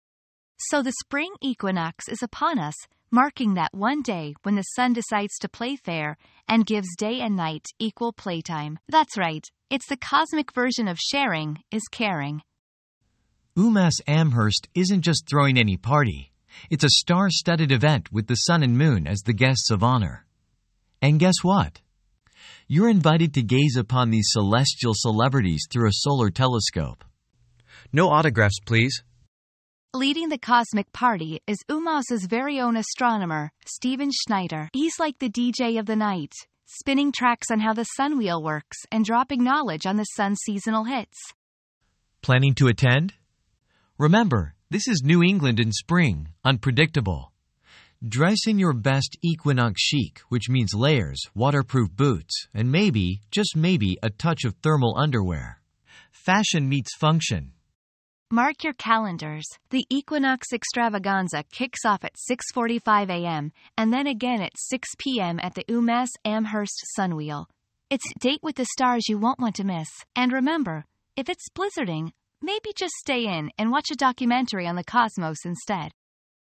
Spring Has Sprung! UMass Amherst's Equinox Extravaganza
Join us as we dive into the first day of spring at UMass Amherst with astronomers, telescopes, and a splash of humor. Who knew science could be this fun?